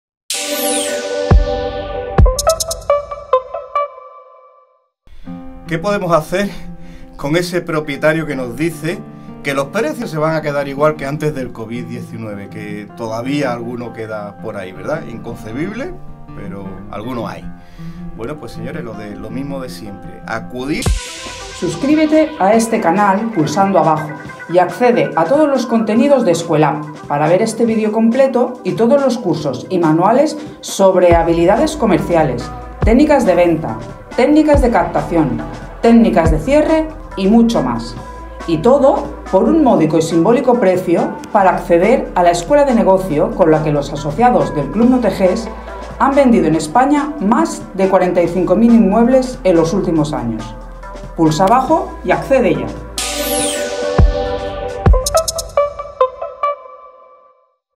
VB43 LOS PRECIOS SE VAN A MANTENER TRAILER
¡Hola!

¿Qué se le responde a un propietario que no se ajusta al mercado porque está convencido que los precios se van a mantener? Descúbrelo en este nuevo VideoBlog de Escuelapp.

Feliz miércoles.